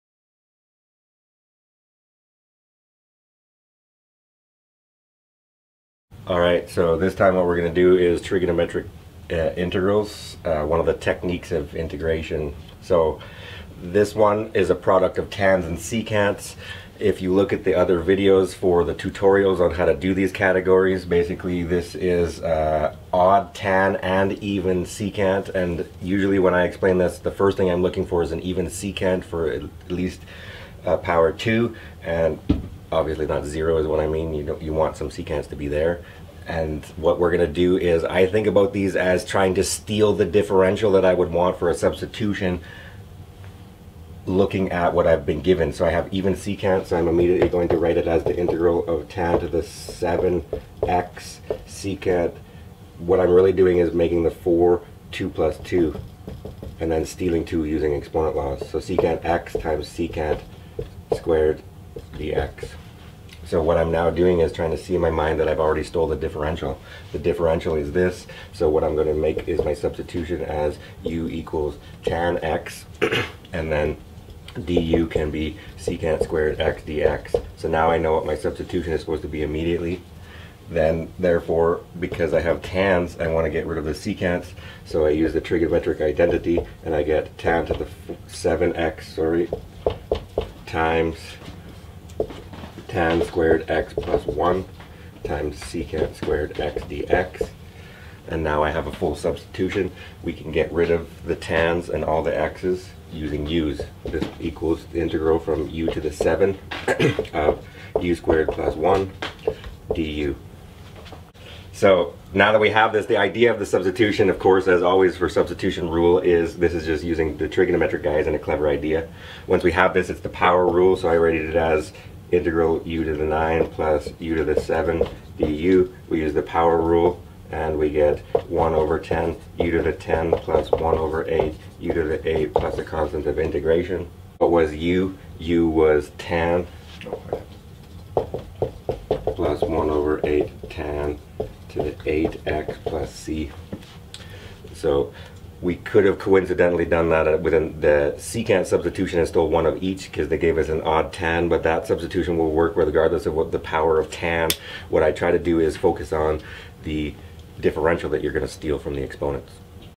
Trigonometric Integration 1
The Tutor Wizard performs integration on products of trigonometric functions!